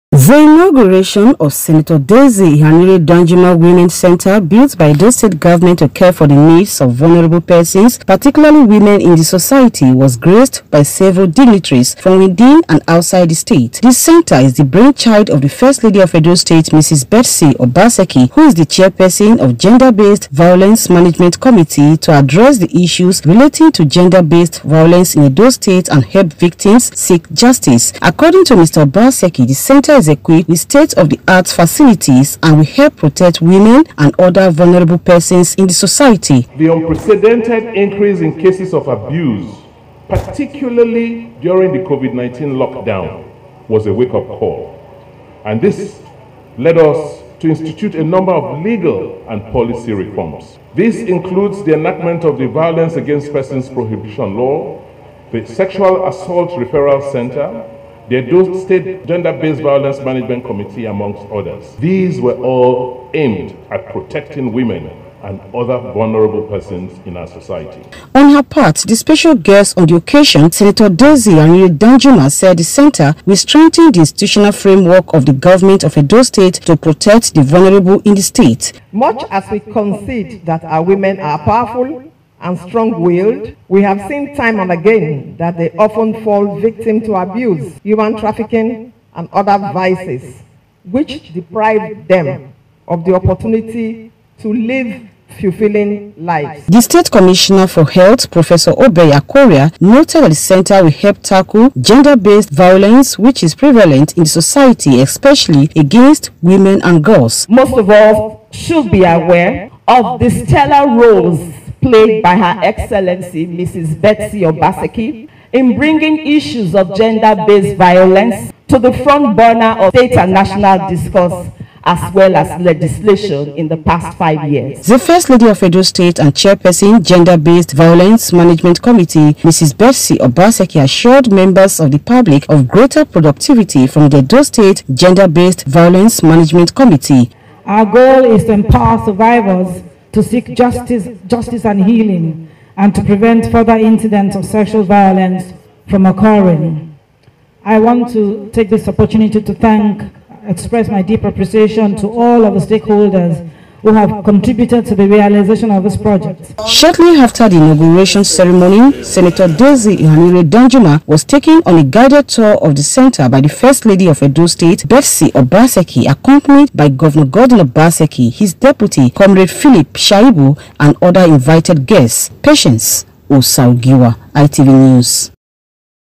Sen. Daisy Ehanire Danjuma inaugurates Women Center named after her
It was a pomp and ceremony during the inauguration of Senator Daisy Ehanire Danjuma Women Center built by the government of Edo State to provide shelter for vulnerable women, empower survivors to seek justice and healing and prevent further incidents of sexual based violence and abuse.

The inauguration ceremony was graced by notable personalities including international partners, traditional rulers, and top government functionaries.

Government House Correspondent, Patience Osarogiuwa has details.